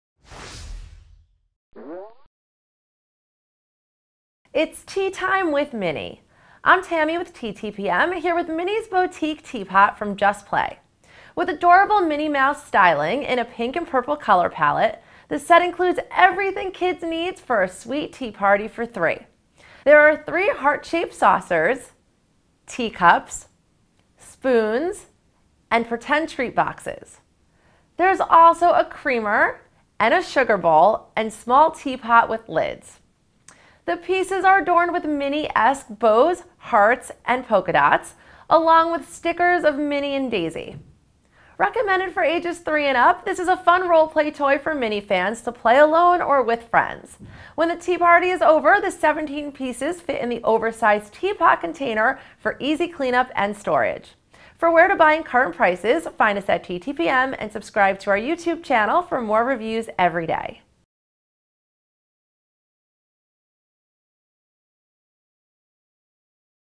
Minnie's Bowtique Teapot from Just Play
NEW 2015 Minnie's Bowtique Teapot to have a tea party. Includes heart-shaped saucers, teacups, spoons, creamer, sugar bowl, teapot, and more. Decorated with Minnie and Daisy.

Product Info: Have a tea party with Minnie's Bowtique Teapot. With adorable Minnie Mouse styling, the set includes everything kids need for a sweet tea party for three.